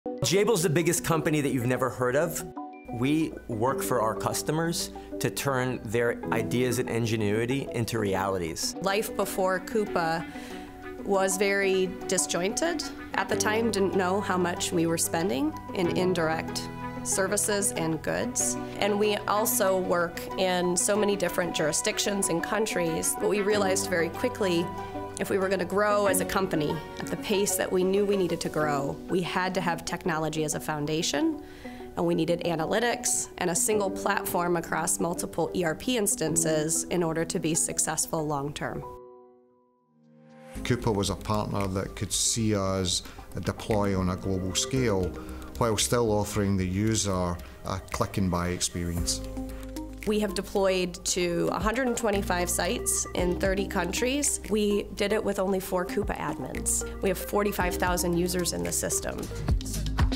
Jabil's Success Story
Discover how Coupa is playing a critical role in helping Jabil grow as a company long term at a healthy and steady pace.